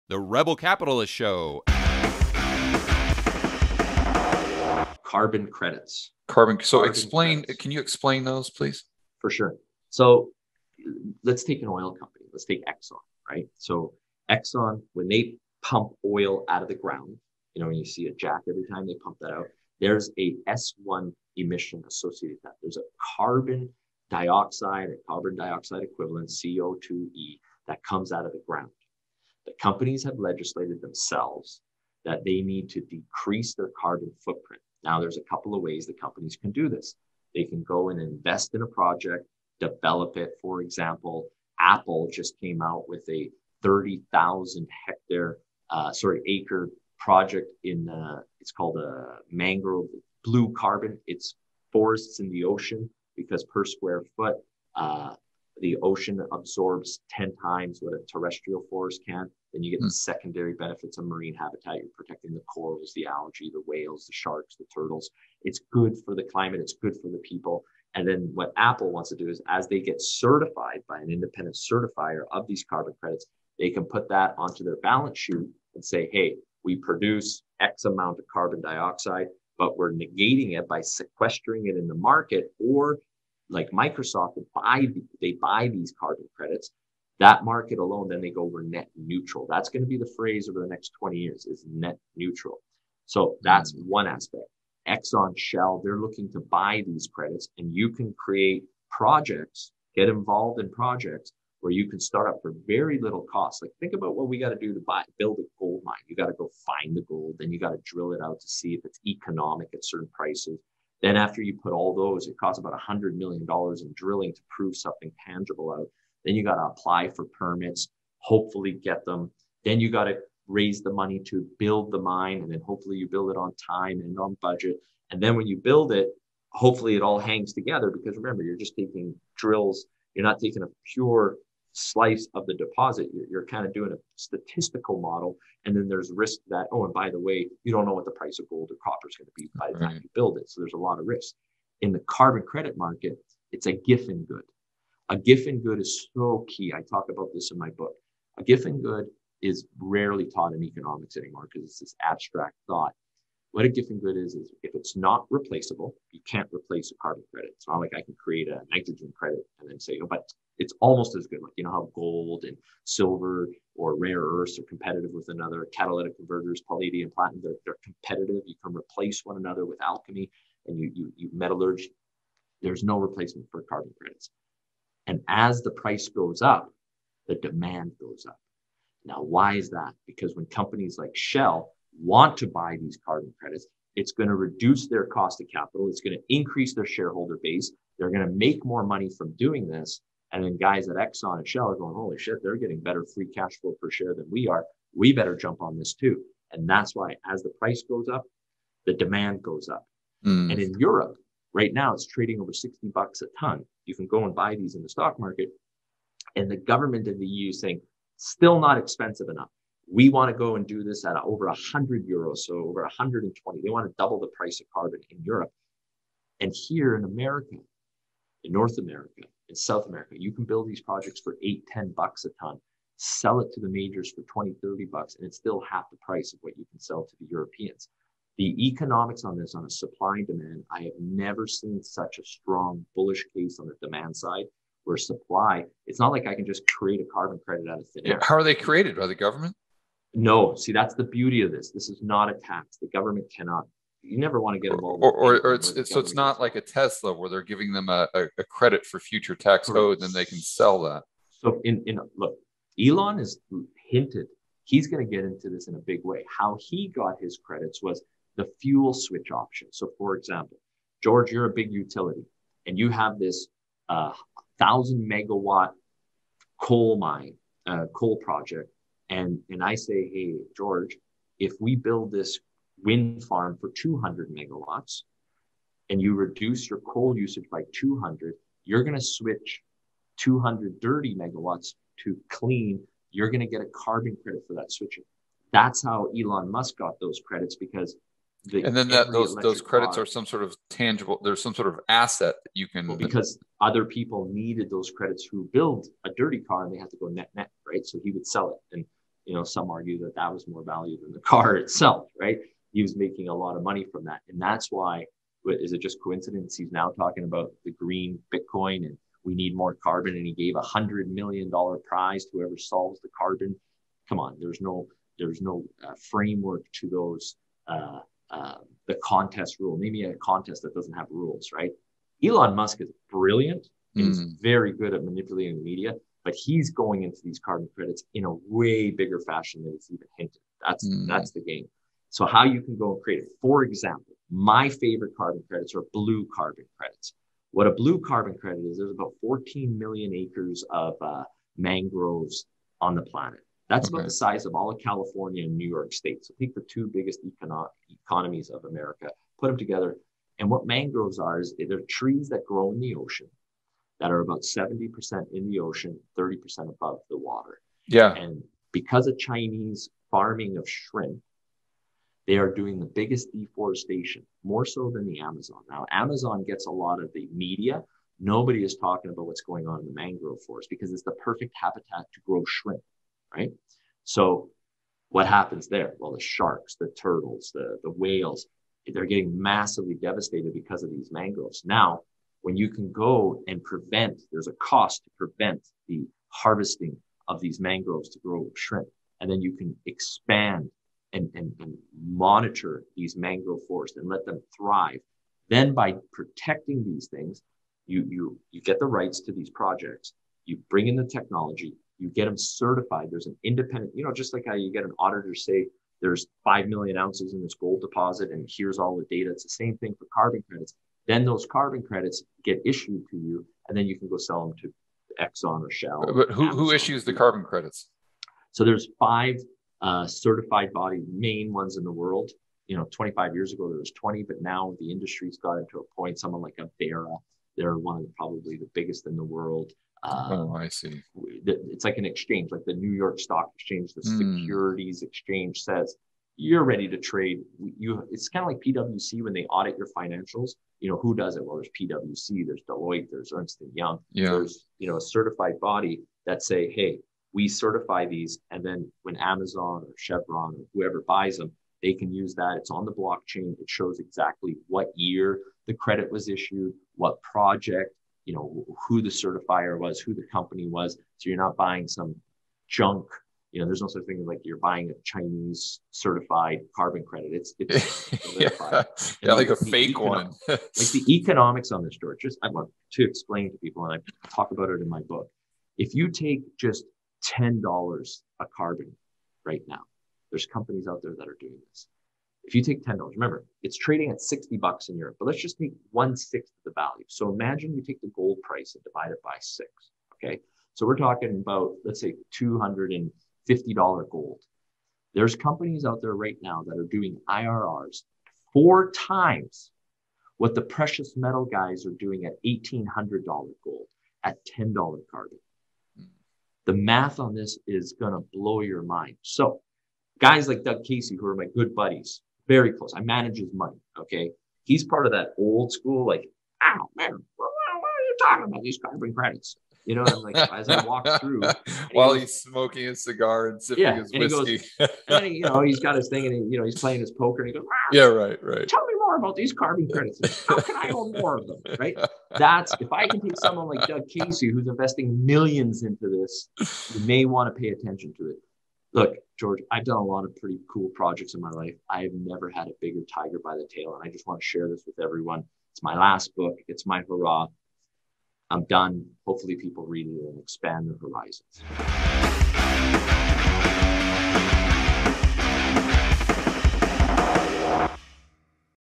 Marin Katusa Talks Carbon Credits, Carbon Footprint, Green Projects, Getting Certified (RCS 166)
Interview original date: June 1st, 2021

Topics- Carbon credits explained: Oil company example. Carbon Dioxide, decreasing carbon footprint, green projects, getting certified, including carbon footprint reduction in balance sheet, cost of production, risks. Carbon credits are Giffen Goods: non replaceable, as price goes up demand goes up, reducing cost of capital, free cashflow. How are carbon credits created? Blue carbon credit, cost prevention, getting the right to certain technologies. Who issues the carbon credits? 5 certified bodies in the world.